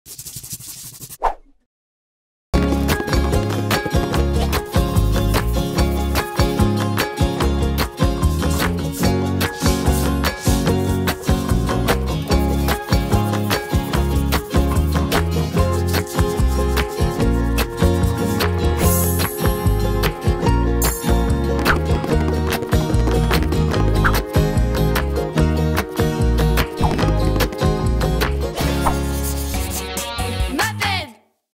Maped Creativ - Aqua'Art - Coffrets de peinture aquarelle enfants
Aqua'Art est une gamme de coffrets uniques qui permet aux enfants de découvrir la peinture aquarelle en toute simplicité.
Ils colorient les tableaux métallisés avec les crayons de couleur aquarellables puis utilisent le pinceau à eau pour peindre avec un superbe effet aquarelle.